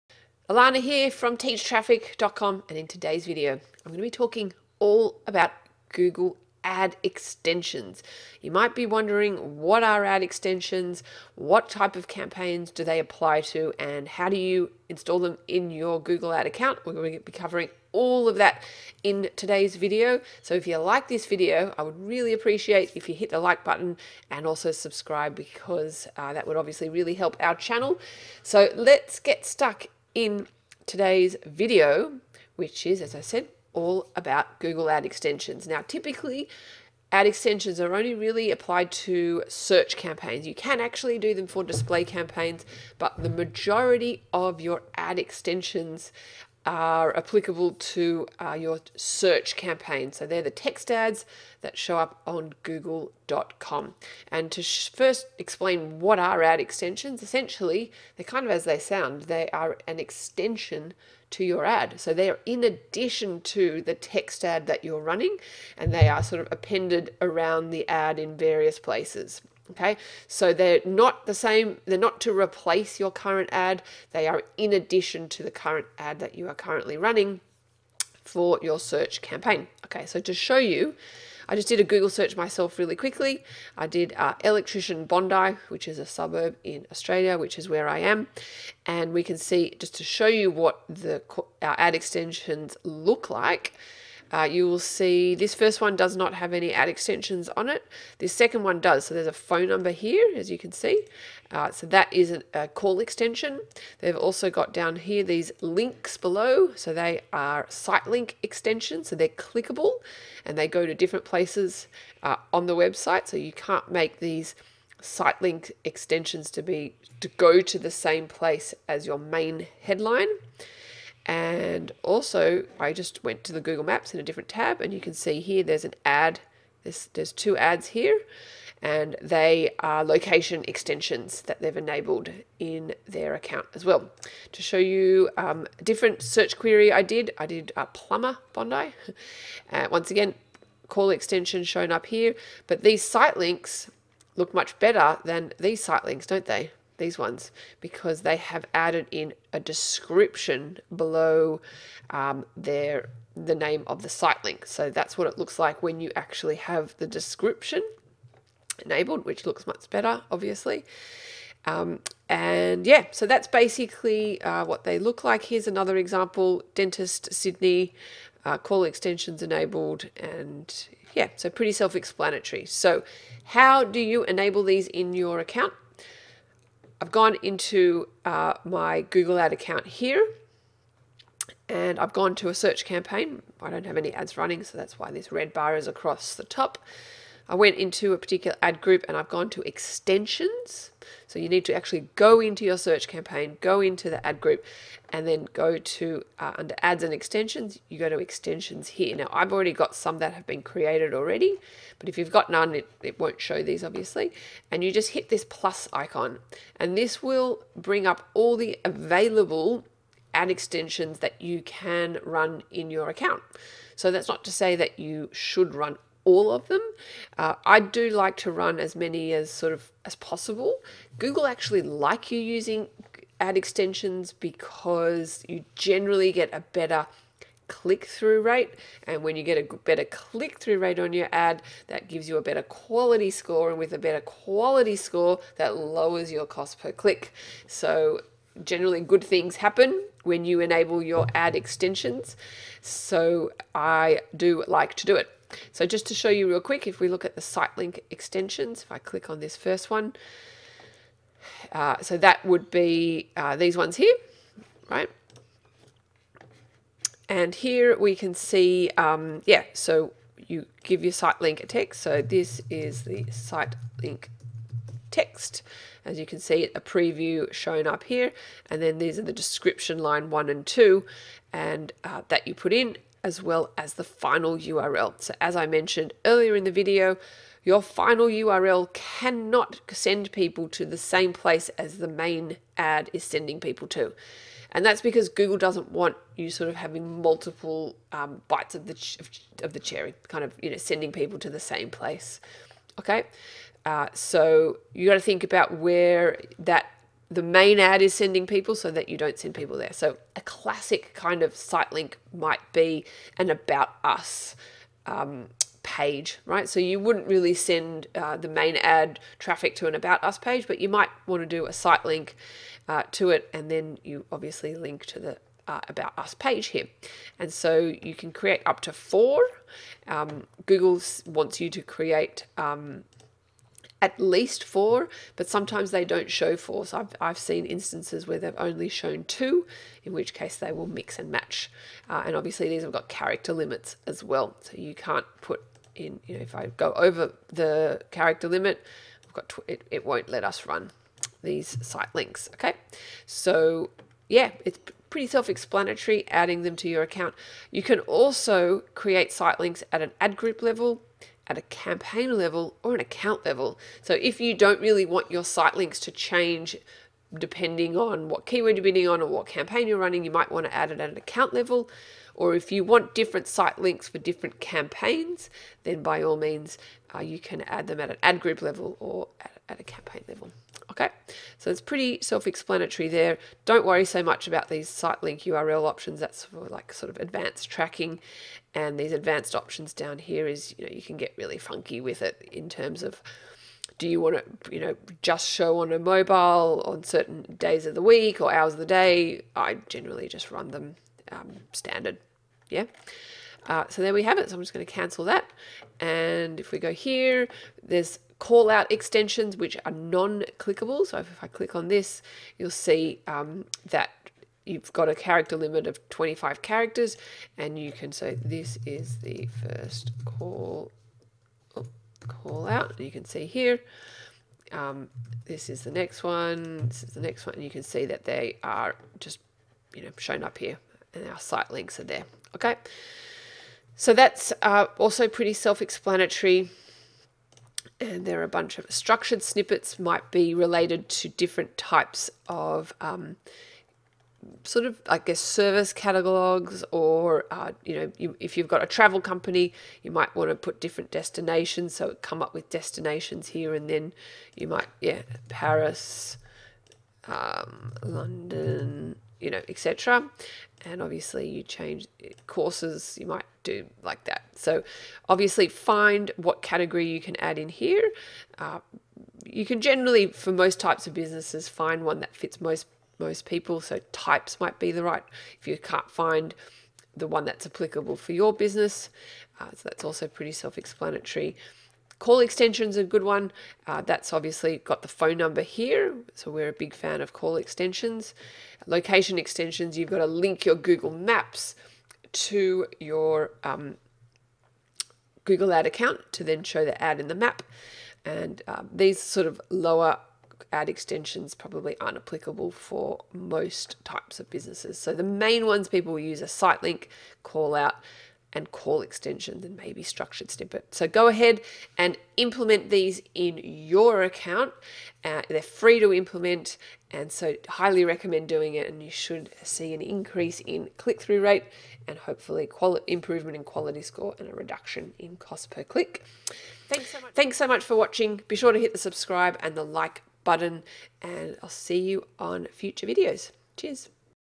Google Ads Extensions Guide 2022 - All You Need to Know!
Google Ads Extensions Guide 2022 - All You Need to Know! - in this video, I will discuss what are Google Ads ad extensions such as sitelinks, location extensions, callout, and more. And I'll be showing as well what it looks like in live ads, so be sure not to miss this video!